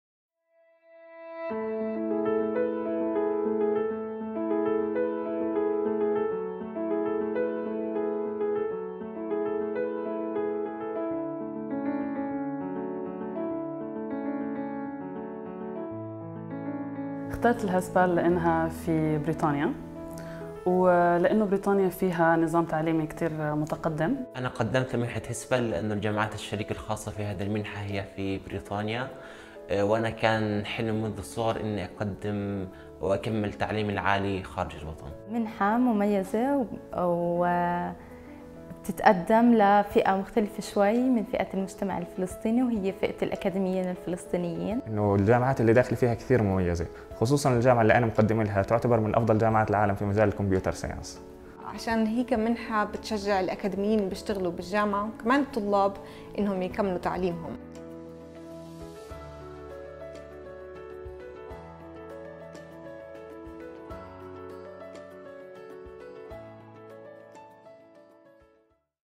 Why did you choose HESPAL?
An interview with some of HESPAL scholars answering the question of "Why did you choose HESPAL?"

HESPAL Higher Education Scholarship Palestine ims to help create the next generation of senior academics who can maintain international quality standards at Palestinian universities and to develop renewed, sustainable links between Palestinian and UK universities.